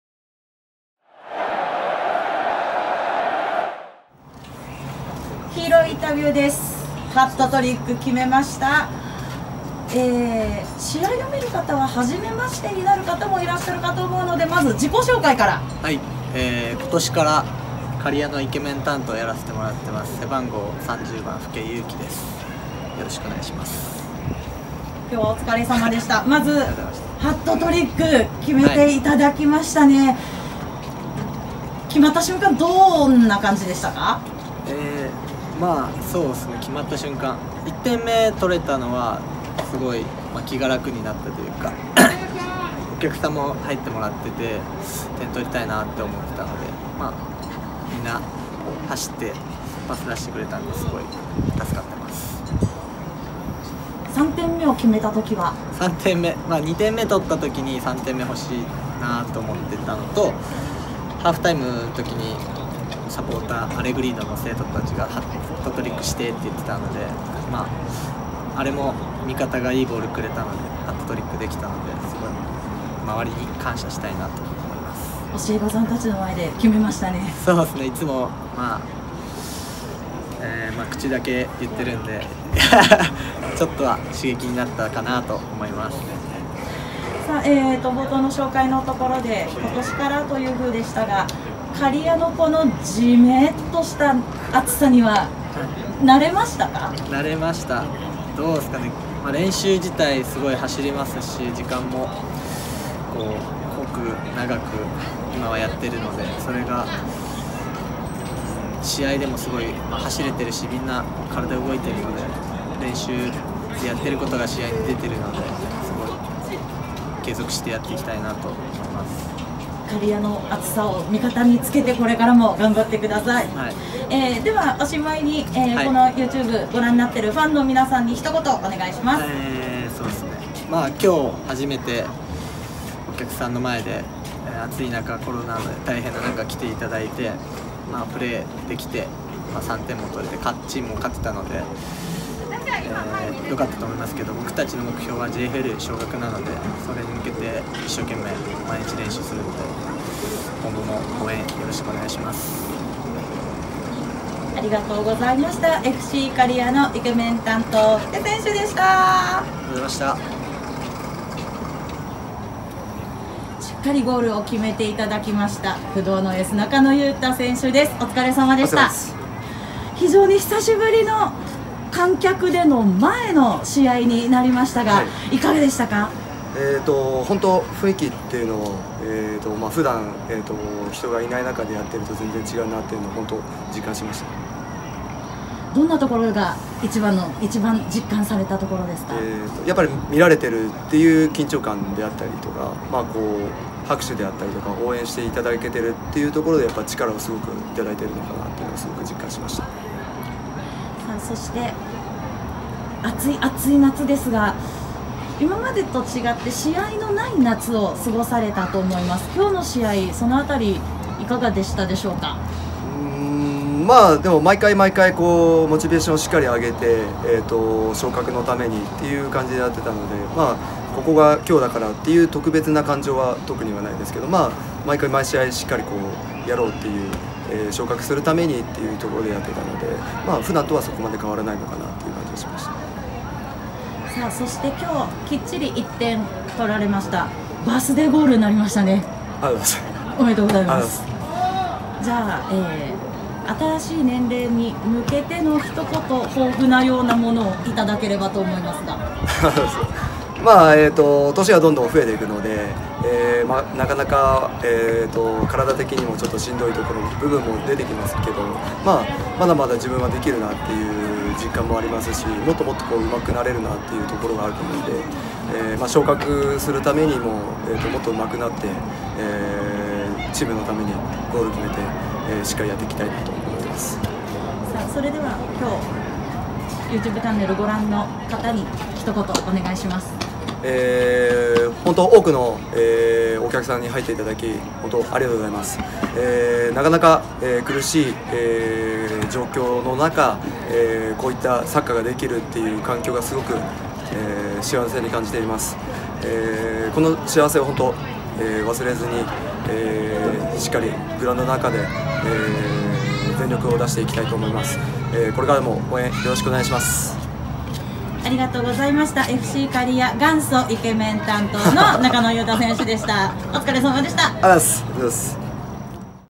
【FC刈谷】2020年8月30日 福家勇輝選手 中野裕太選手 試合後ヒーローインタビュー
2020年8月30日、ウェーブスタジアム刈谷にて行われた、刈谷ダービーマッチ　AS刈谷戦　のヒーローインタビューをお届けします！

00:00　福家勇輝選手インタビュー
02:42　中野裕太選手インタビュー